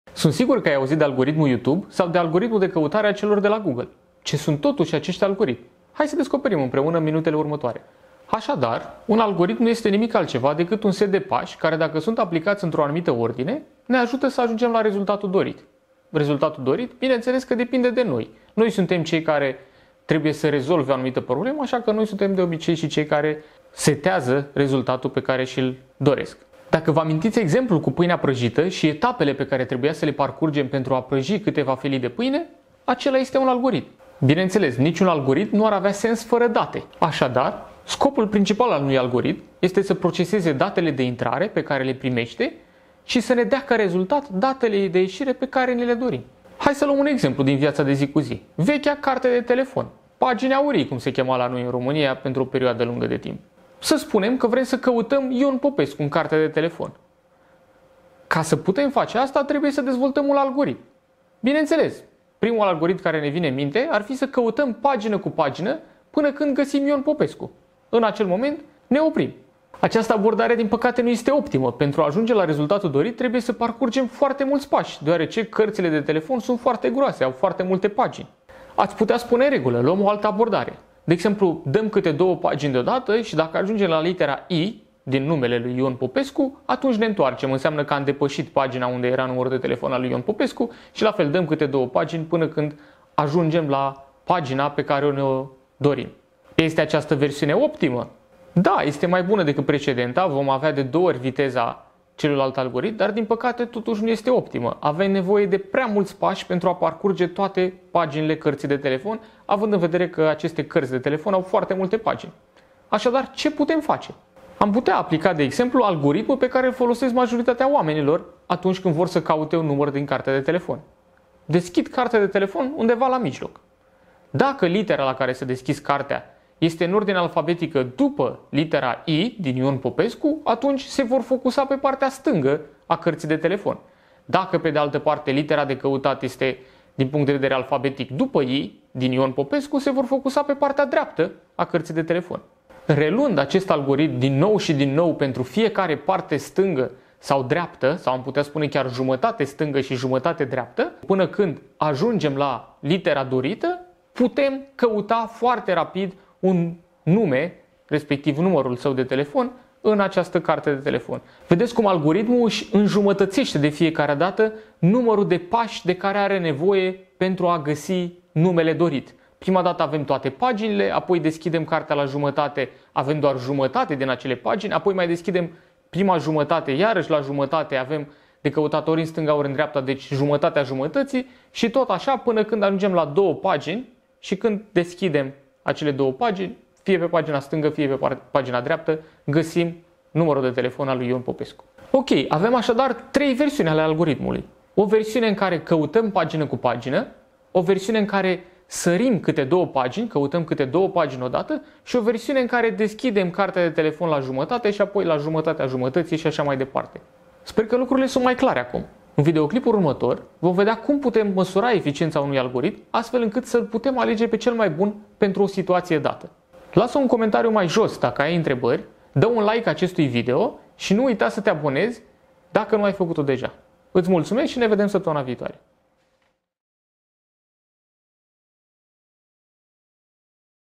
Algoritmii: ce sunt și de ce avem nevoie de ei?🚦| Episodul 8
În episodul de astăzi discutăm despre algoritmi. Cu toții am auzit de algoritmul YouTube sau de algoritmul de căutare al celor de la Google. Ce sunt acești algoritmi? Discutăm detaliile în episodul de astăzi. Abia aștept întrebările tale în zona de comentarii 🧐